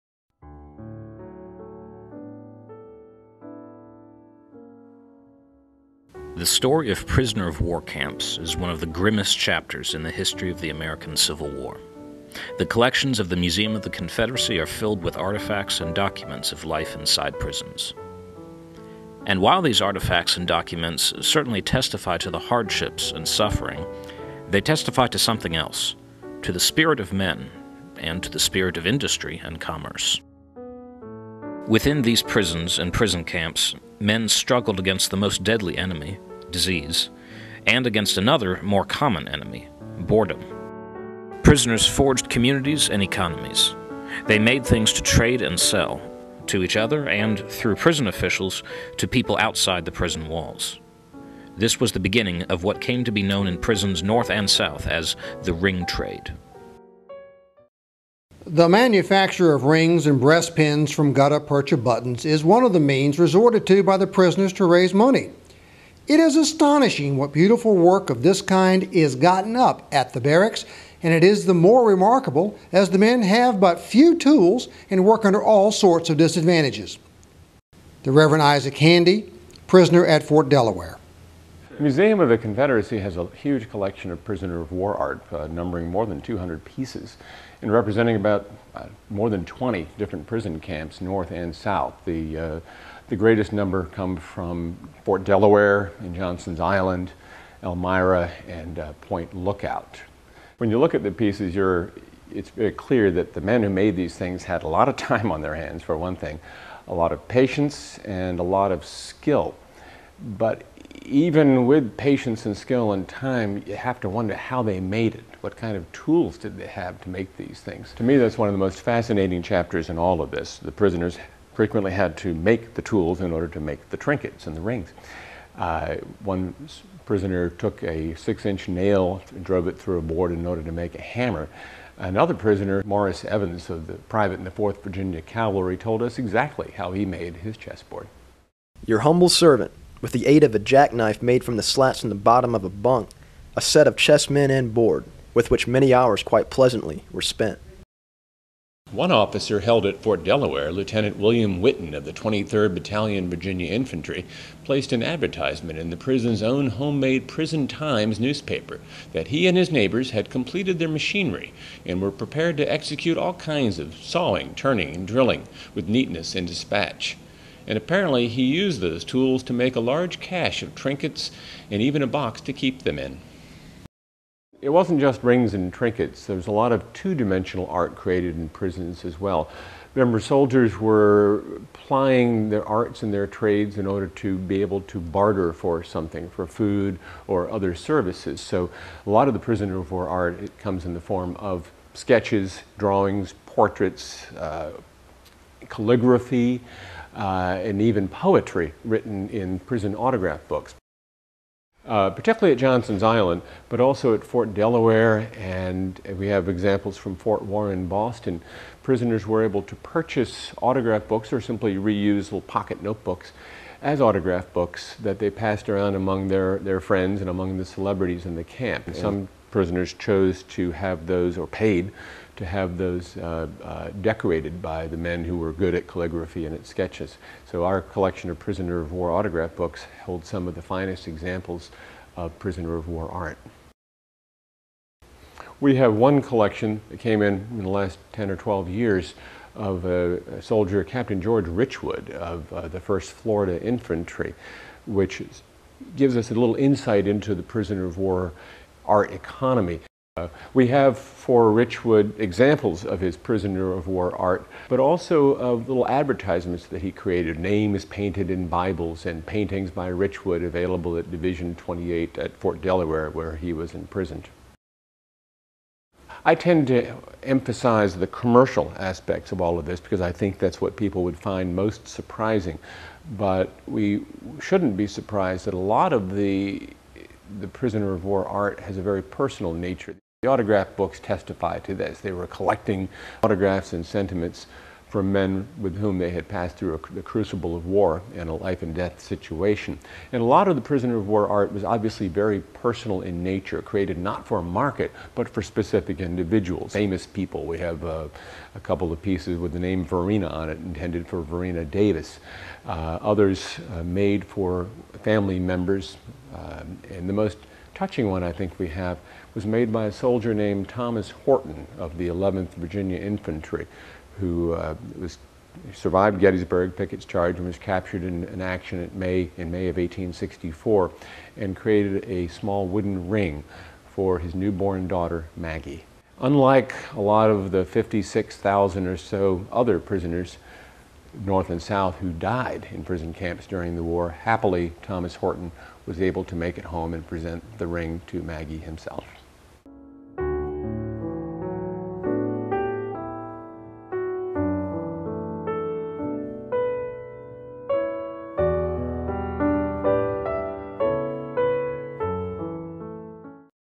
Creativity in Captivity: The Museum of the Confederacy's Prisoner of War Art Collection
Prisoners of war during the American Civil War faced many hardships. They battled scarcity, disease, and more commonly, boredom. Some industrious prisoners took on all of these challenges by making, trading, and selling various trinkets or works of art. This video takes a look at many of these artifacts from the Museum of the Confederacy's collection and discusses how they testify to the spirit of men and the spirit of industry and commerce.